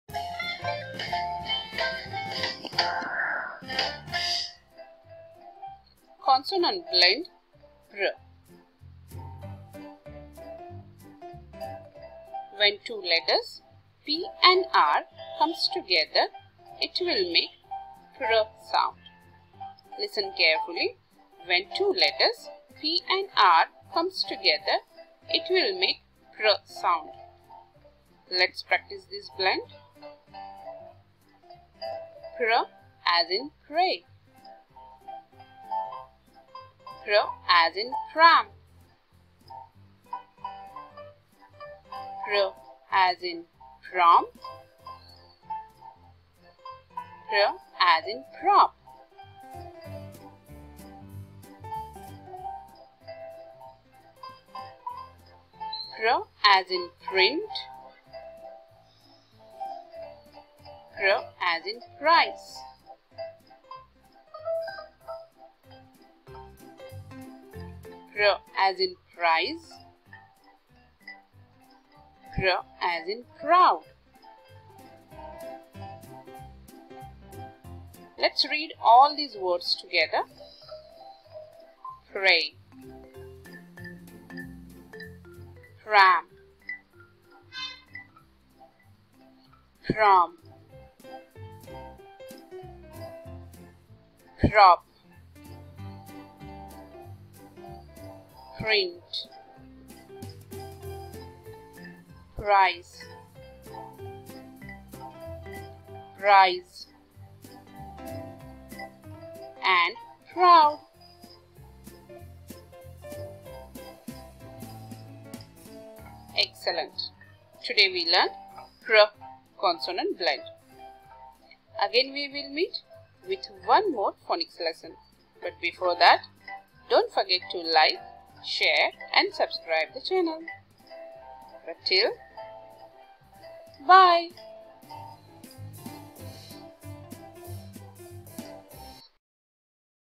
Pr - Consonant Blend
This Video helps student to learn and practice pr consonant Blend.
.Kids Kids Study Station English Reading Phonics Parents#